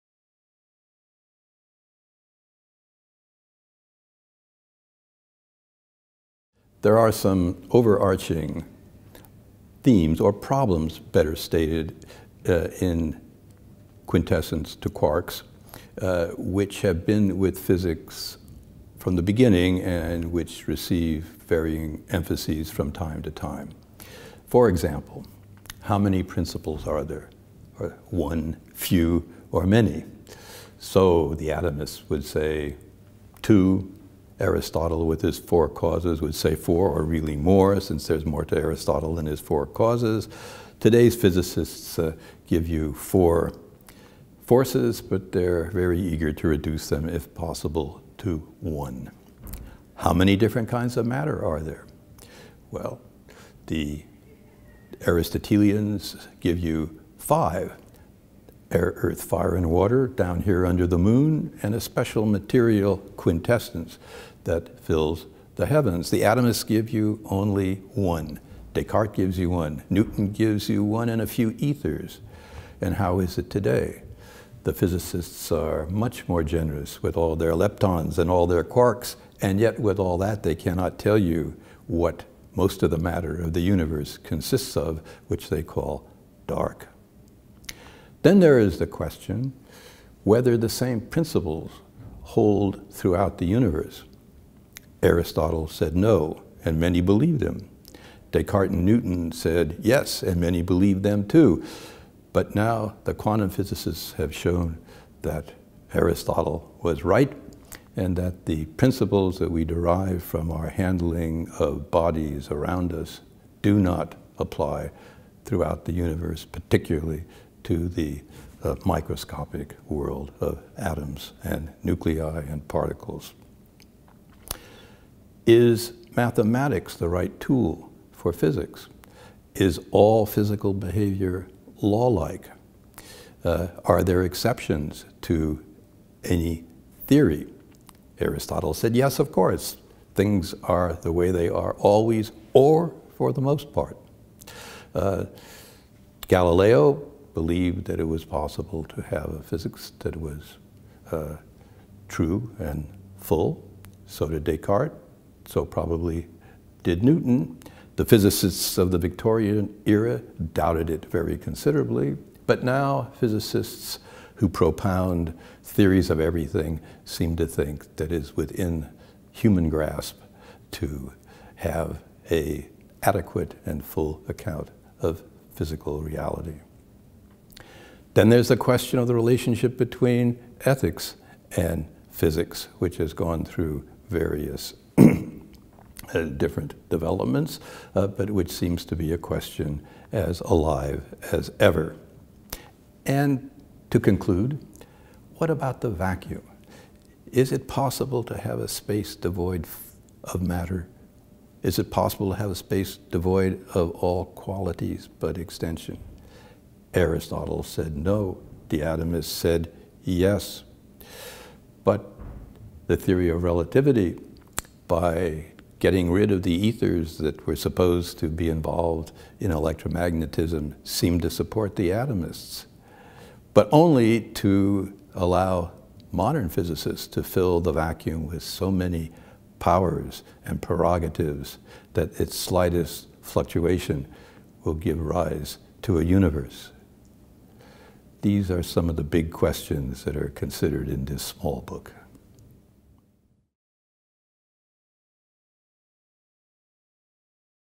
John Heilbron’s lightning tour of the big questions in physics
Author John Heilbron explores the questions that have concerned Physicists since the origin of the study in Ancient Greece.  From ‘how many principles are there?’ to ‘can there be such thing as a vacuum?’, Heilbron traces the changing place and purpose of physics by those that have nurtured it over the centuries.

John Heilbron was educated at the University of California, Berkeley, in physics and history.
He returned to Berkeley in 1967, where he rose to become professor of history and vice chancellor. In recent years he has taught at Caltech and Yale, and he is currently Senior Research Fellow at Worcester College, Oxford, and the Oxford Museum for History of Science. His many books include the biography Galileo, and Love, literature, and the quantum atom.